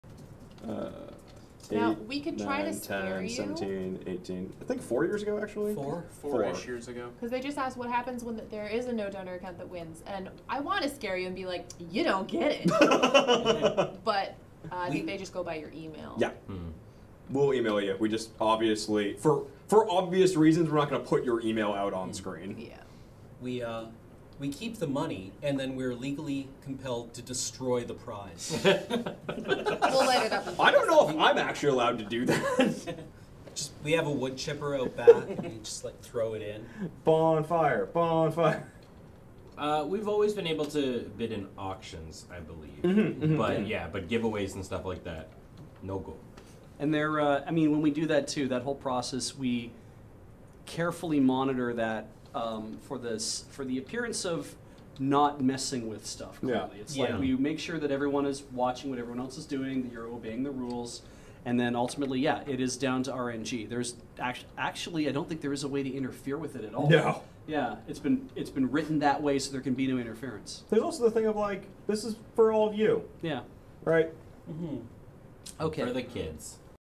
DB2018 - What happens if you don't have a donor account and why the crew can't enter giveaways
"If you don't give us your address we'll destroy the prize" and why the crew can't enter giveaways